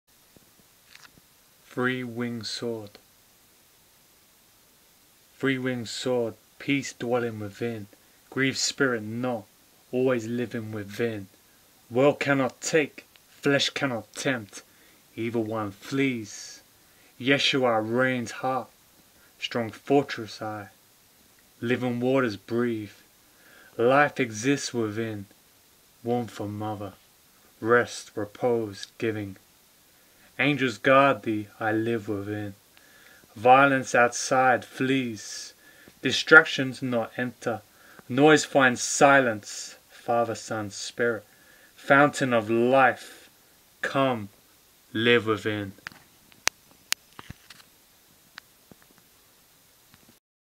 Three Winged Sword
Title: Three Winged Sword

Grieve Spirit not

World cannot take

Flesh cannot tempt

Evil one flees

Yeshua reigns heart

Strong fortress I

Living Waters breathe

Life exists within

Warmth of mother

Rest, repose giving

Angels guard thee

I Live within

Violence outside flees

Distractions not enter

Noise finds silence

Father Son Spirit

Fountain of Life

Come live within